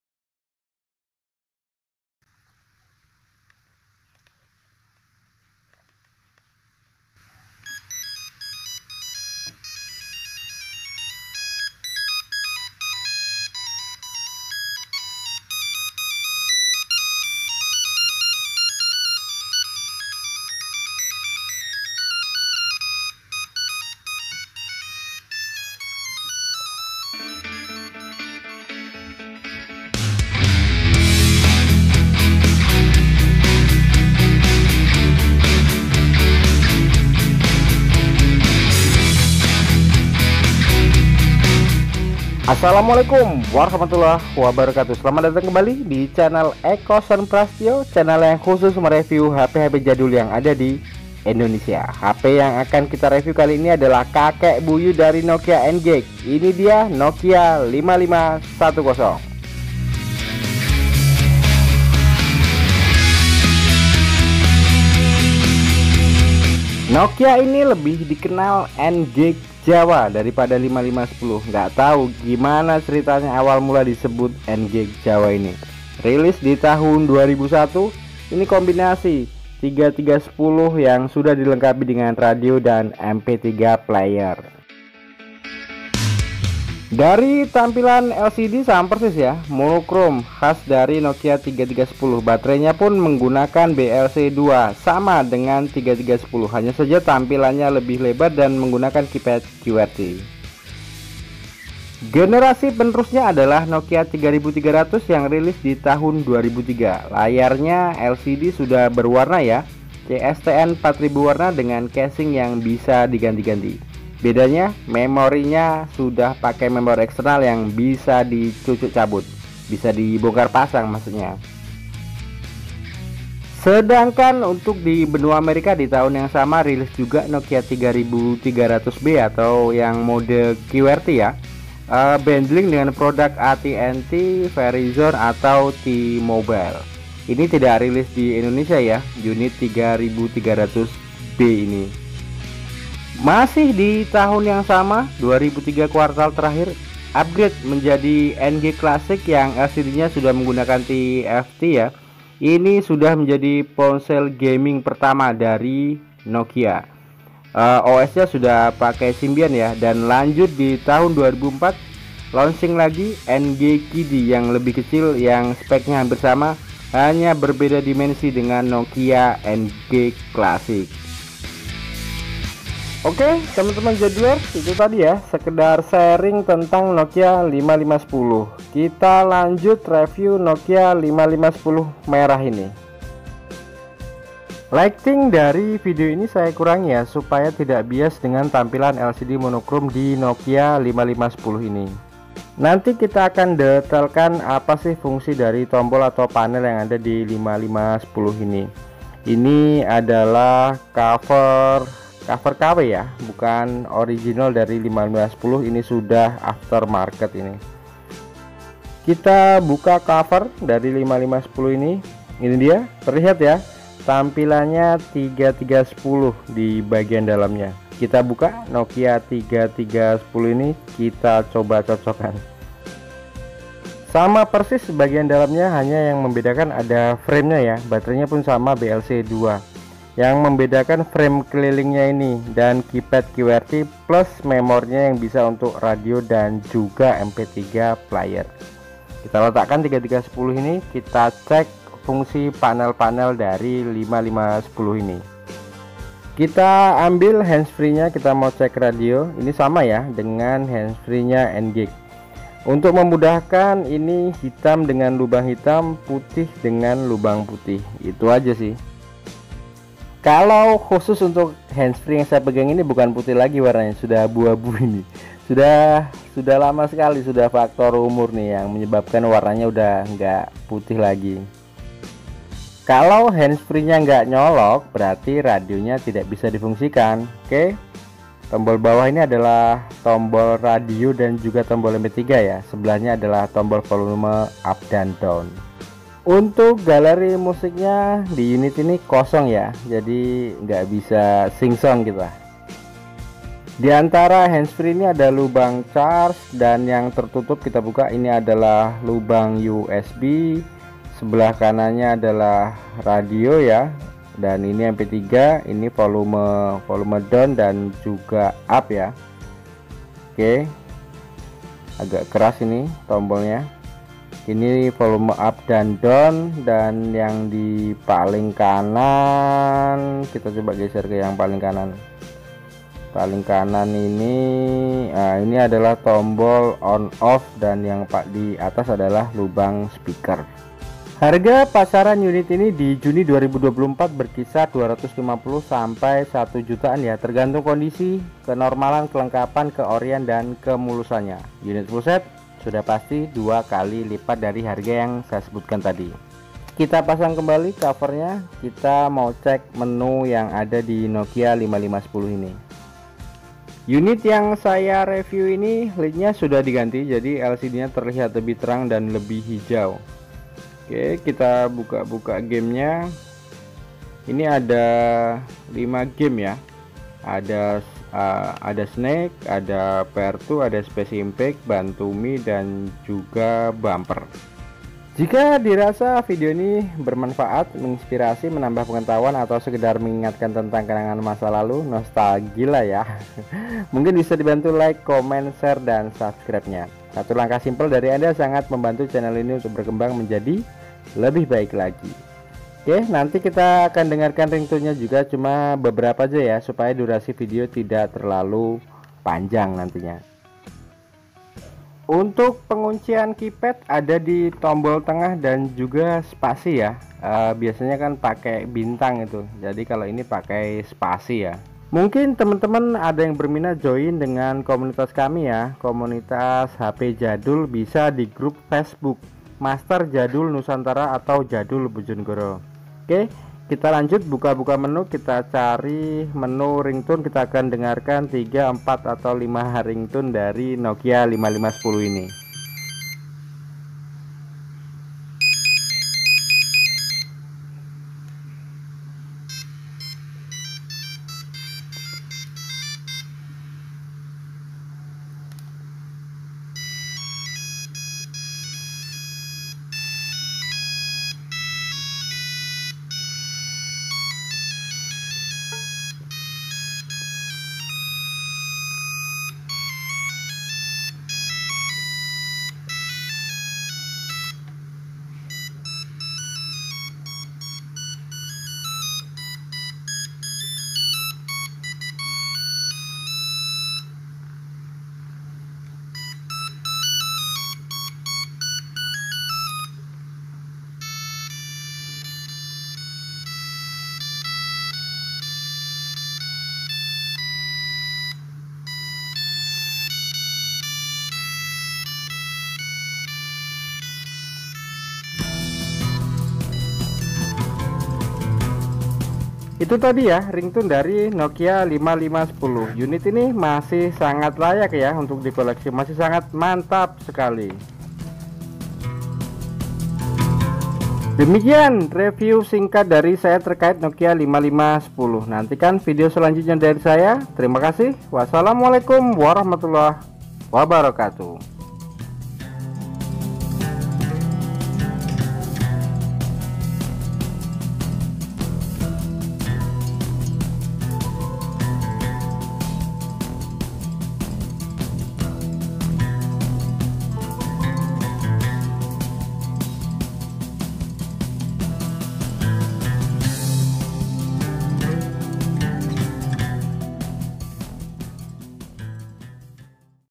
NOKIA 5510 | (sering di sebut juga) N GAGE JAWA | HP JADUL BOJONEGORO
Untuk contact Kami tentang unit yang di review,
kritik,Saran,Penjualan atau pembelian unit jadul
silahkan ke chat WA: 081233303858

Detail specification nokia 5510 QWERTY

Launch 2001
GSM-mini SIM
Colors 2 - Groove Red and Melody Blue
Radio Stereo FM radio
MP3 player and recorder

Dimensions 134 x 58 x 28 mm, 165 cc (5.28 x 2.28 x 1.10 in)

Messaging SMS
Browser WAP
Clock Yes
Alarm Yes
Games 5
 ( Snake II, Pairs II, Space Impact, Bantumi, Bumper )
Languages 23

Smart messaging
Calculator
Voice Dial
Mobile chat
Profiles
Currency converter
DISPLAY
Type        Monochrome graphic
Resolution 5 lines
 Dynamic font size,Softkey,Screensavers,Welcome message

Alert types Vibration; Downloadable monophonic ringtones
6 Ringing tone levels
10 Volume levels
Up to 10 h MP3 playback
Up to 13 h 30 min FM playback

MEMORY
Phonebook 100
Call records 8 dialed, 8 received, 8 missed calls
Internal 64MB
150 messages or 50 picture messages
Message templates
220 calendar notes
8 voice dial numbers
BATTERY
Type Removable Li-Ion 950 mAh battery (BLC-2)